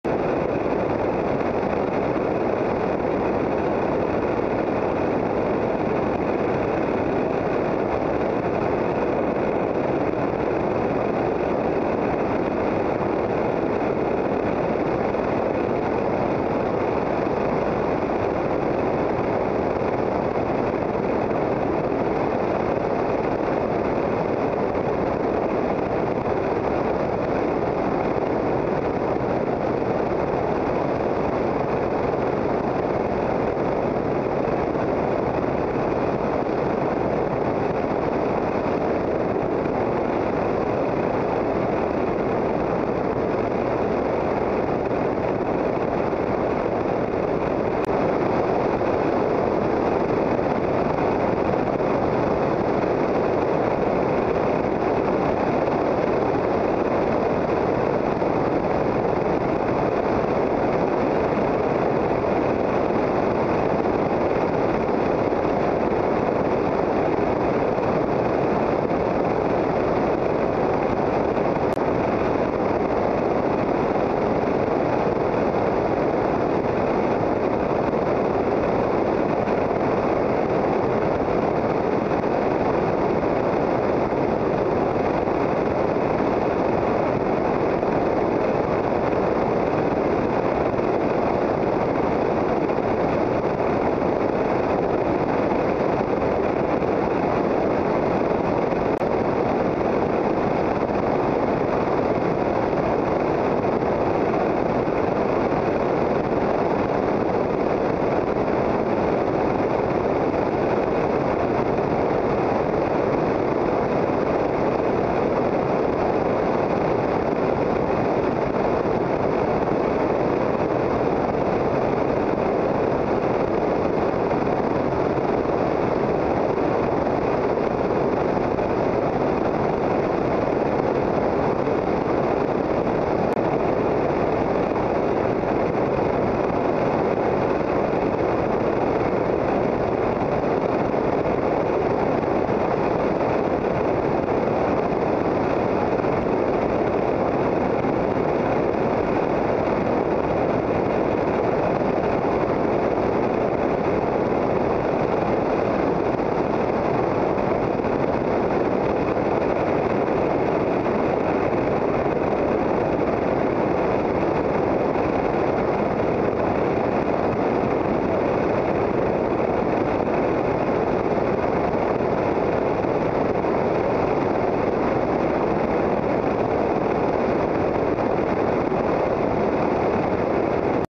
Local Lightwing Flight
A quick preview of the countryside where I live and fly, The first video taken from my lightwing, 28-5-13